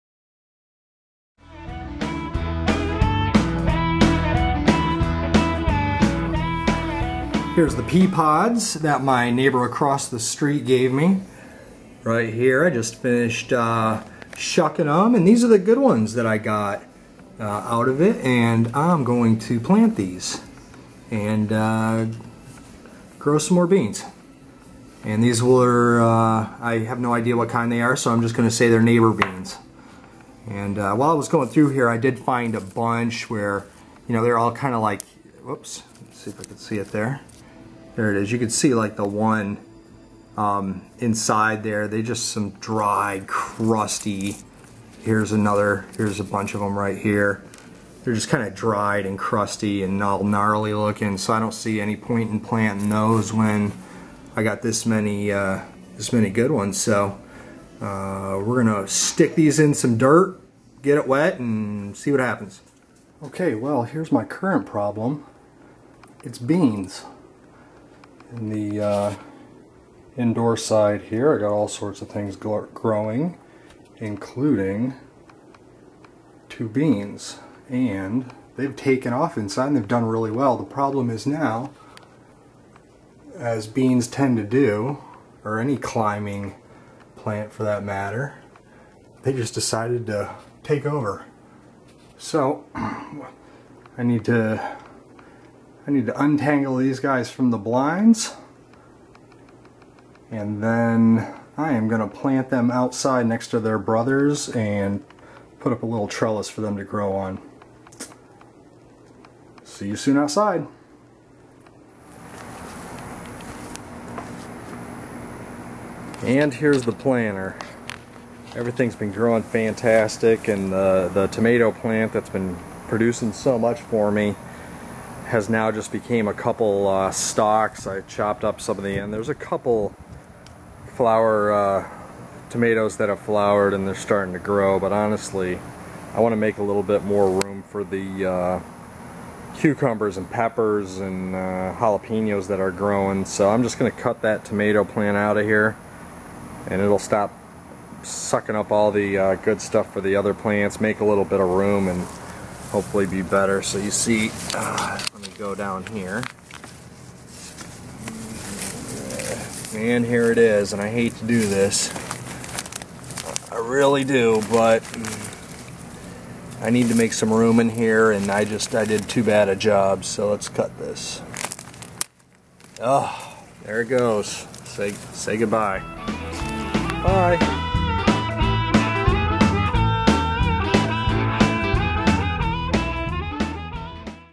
Indoor Bean Vines And Killing A Tomato Plant
Quick video that's going to be leading up to my next tour.

Part 1: Some beans my neighbor had given me
Part 2: Indoor container beans have vines using my blinds for a trellis!
Part 3: I kill my cherry tomato plant that's in my earth box style container so I can have more room, plus the over-pruning I did to it last month is affecting it.